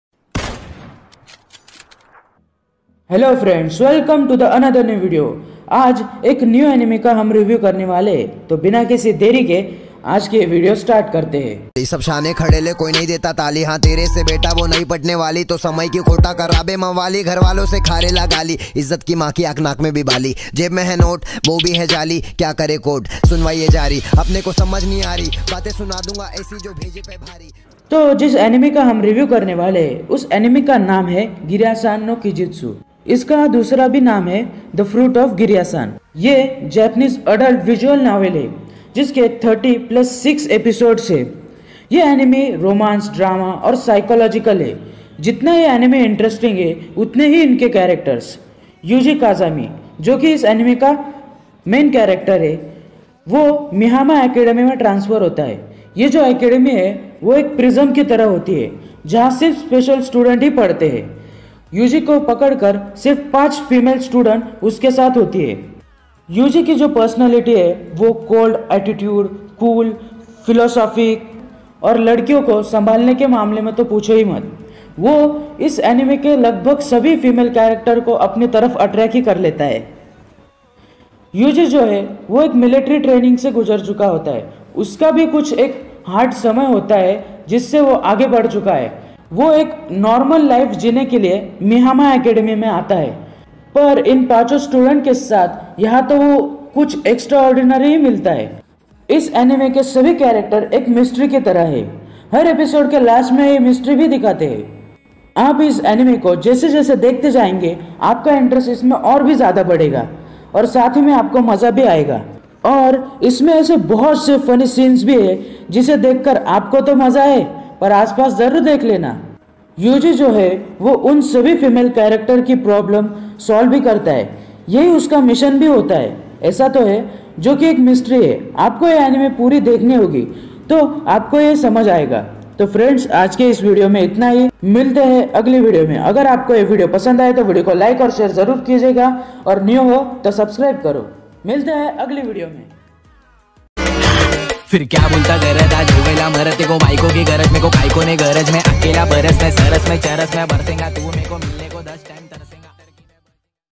The Fruit Of Grisaia - ANIME REVIEW [Hindi]
Title :- The Fruit Of Grisaia - ANIME REVIEW [Hindi]
hello friend, welcome to "Team Anime Addict"
hello friend aaj bhi hum aapke liye ek mast anime leke aaye he...video ko end tak dekhna 😊
🔼About anime:-
⏺Name :- The Fruit Of Grisaia (Grisaia no kajistu)
⏺episodes :- 13
🔼credits :-
⏺- Script by :- Rohan
⏺- Voice by :- Rohan
⏺- Edit by    :- VILLEN

🔼Audio credits :-
⏺NCS

keep supporting 🙏